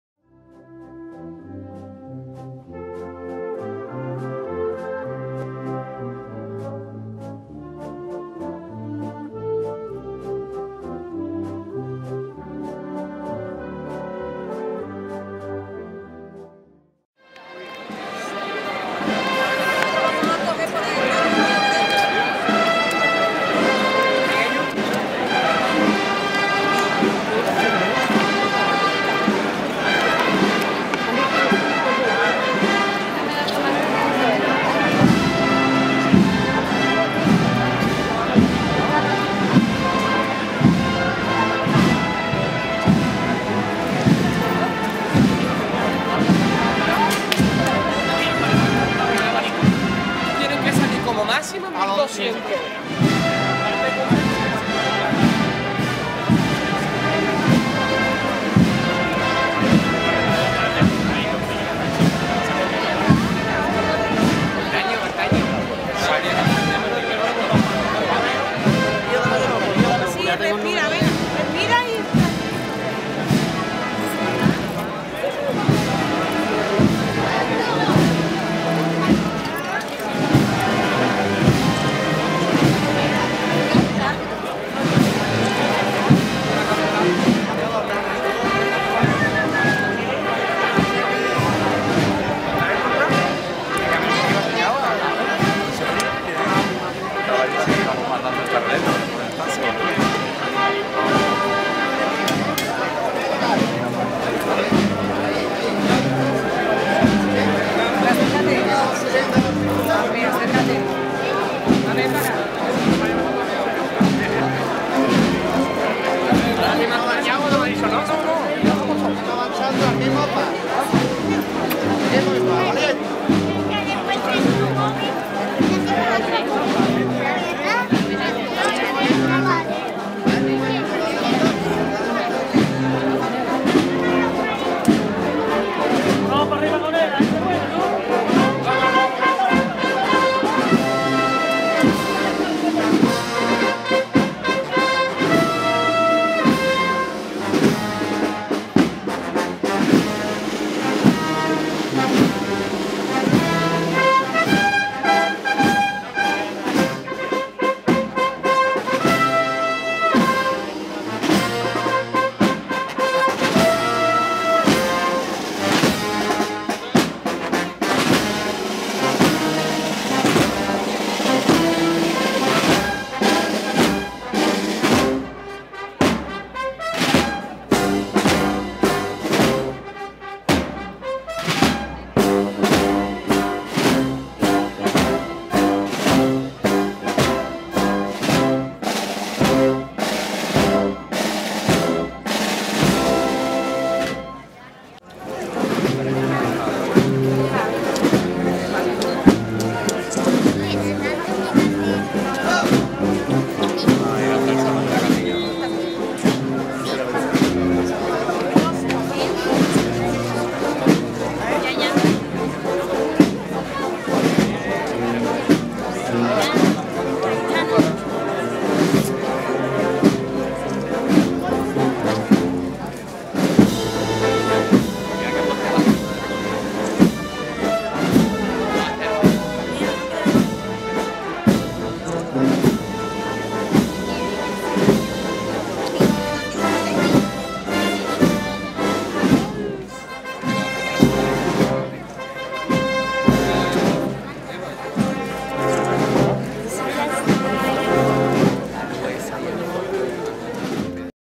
Corpus Christi - Carmen de Triana en el Puente
Graba @Clau_BarTej. Acompaña musicalmente la Banda de Música de Las Cigarreras de Sevilla.